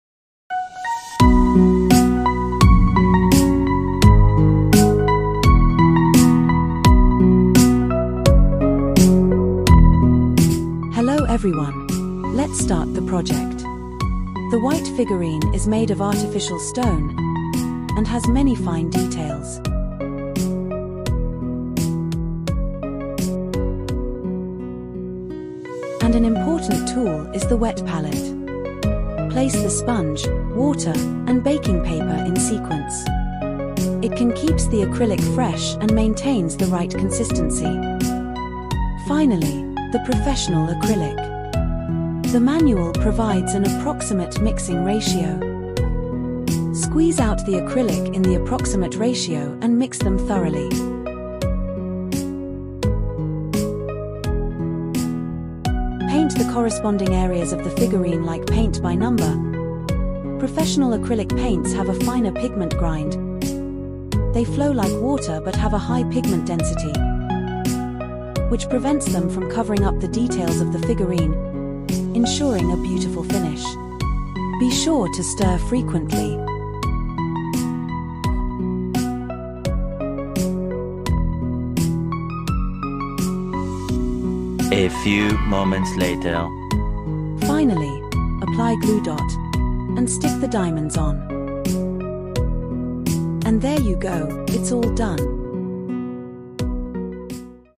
Peacock Figurine Arts and Craft for Adults Tutorial, New Paint by Number DIY Project
How to paint your own peacock figurine with acrylic. New DIY Arts and Craft for Adults.